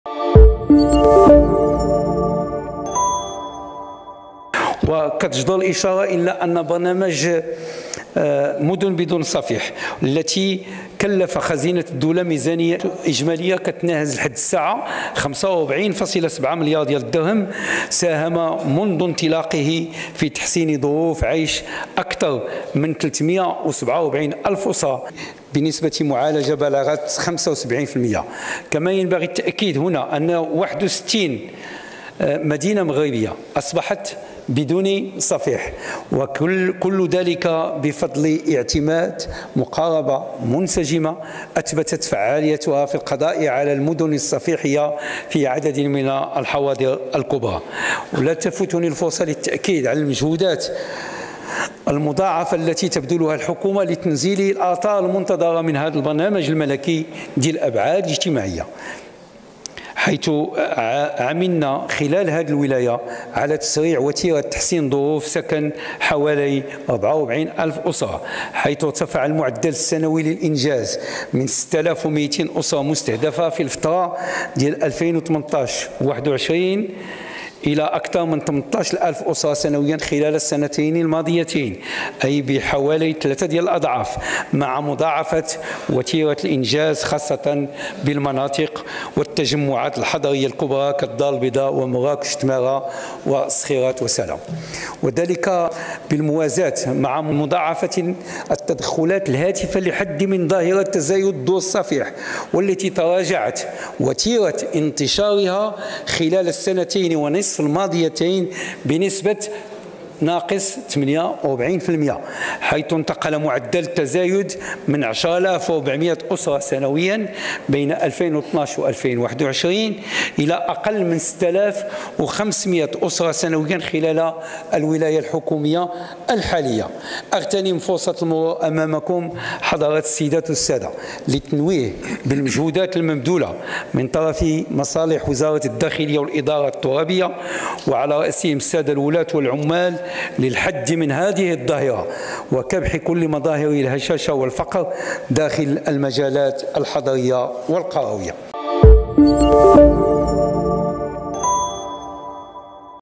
أخنوش: 61 مدينة بدون صفيح وتراجع بنسبة 48% في عامين ونصف
يتحدث عزيز أخنوش، رئيس الحكومة، عن مشروع مدن بدون صفيح والانعكاسات الإيجابية لهذا المشروع على حياة العديد من المواطنين الذين كانوا يستقرون بمجموعة من الكارينات على الصعيد الوطني، مقدما بعض الأرقام حول هذه القضية